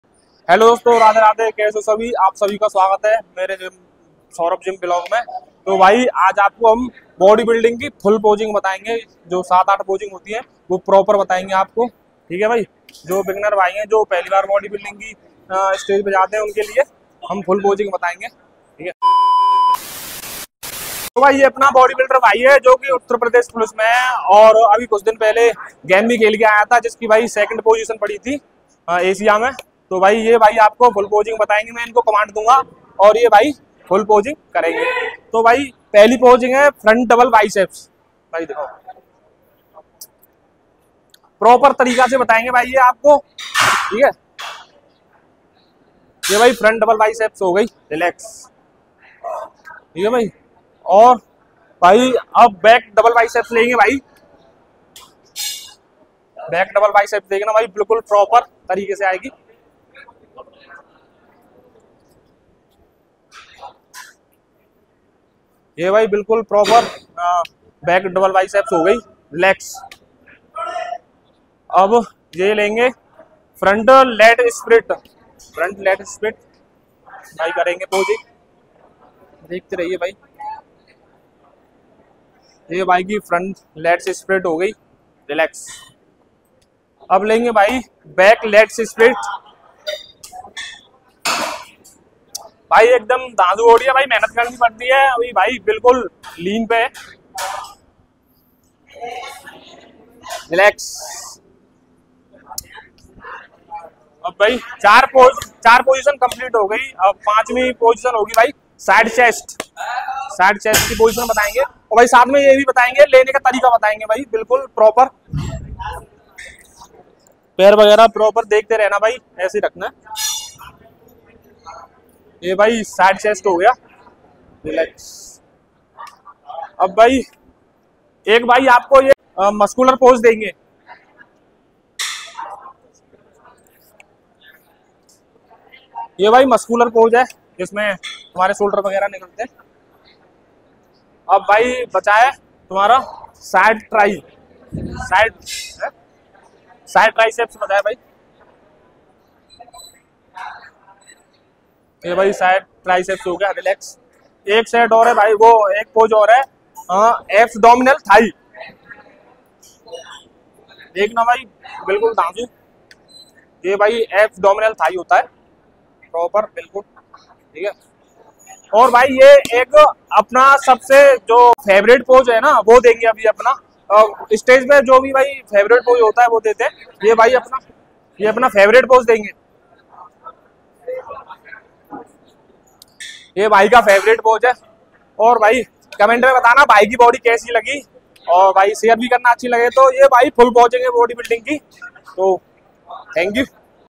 Bodybuilding competition full posing lene ka tareeka
hello dosto kese ho sabhi bhai

 jiss kisi bhai ko bodybulding posing nhi aati h
wo ye video ko dekh kar posing seekh skta h

or Gym se related jayada jankari ke liye
my instagram id  @saurav_singh9290 ko follow kar skte h and dm bhee kr skte h

jayada se jayada video ko piyar dena

thanks 🙏🏼